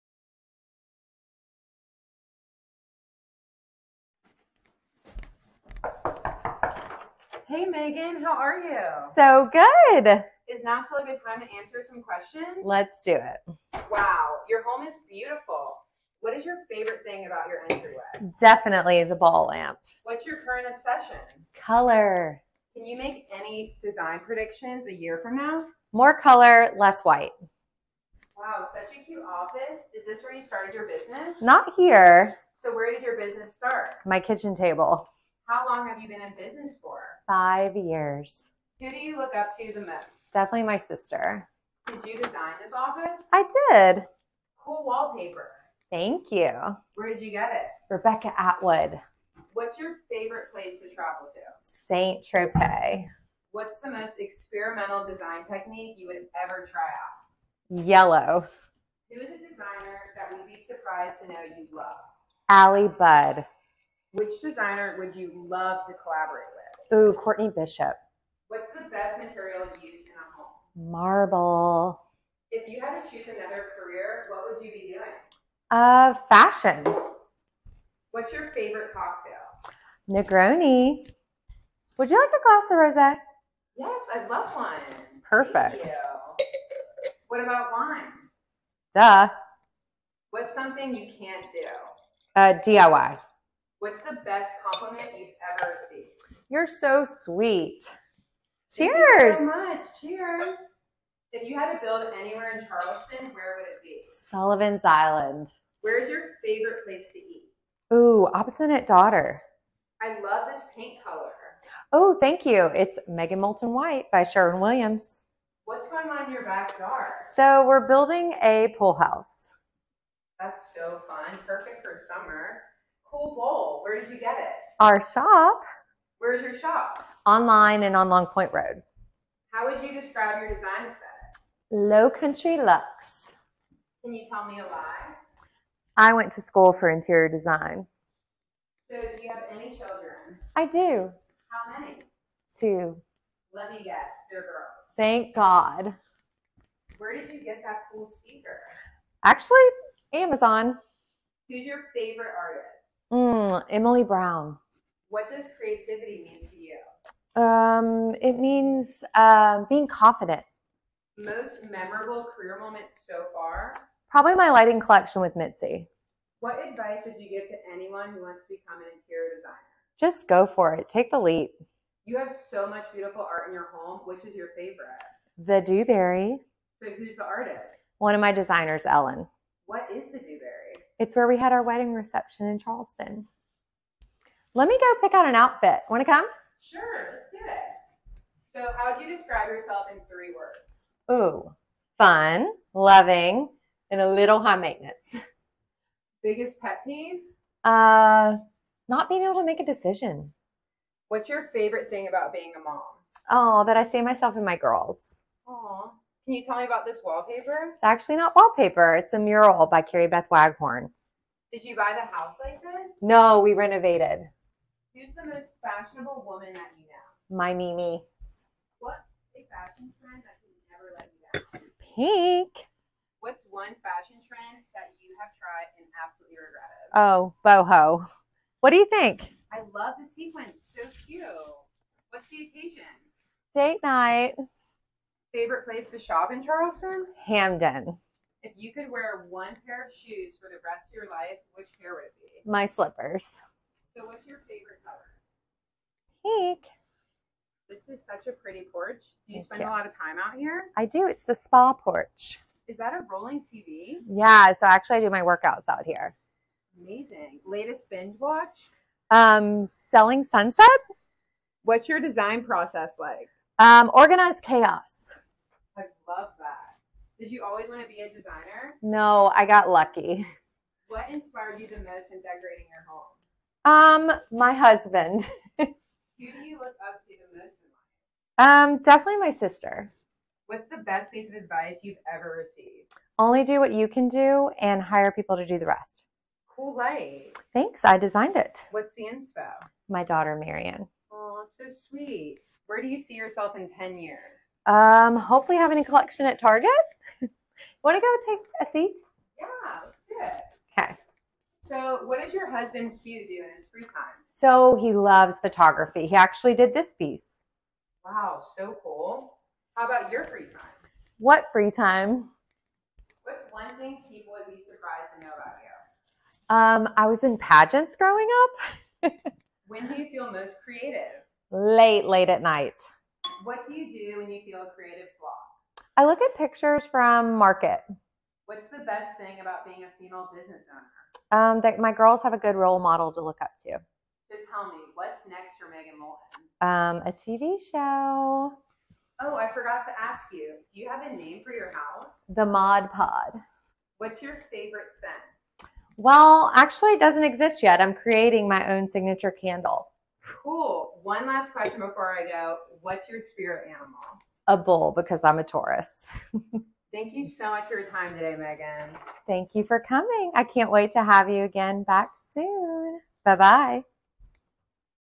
Interior Designer Megan Molten Answers 73 Questions | MEGAN MOLTEN {The Mod Pod}
We got so inspired by the VOGUE 73 Question videos that we decided to create our own version! Interior Designer Megan Molten takes us on a tour of “The Mod Pod,” her renovated home in Mount Pleasant, South Carolina. Megan answers all of our questions, including proudest career moments and where she sees herself in 10 years. Watch as Megan answers 73 rapid-fire questions while taking us on a tour of her beautifully decorated home.

P r o d u c t s    f r o m    o u r     s h o p~

Video: Megan Molten Studio

M o r e    f r o m    M e g a n~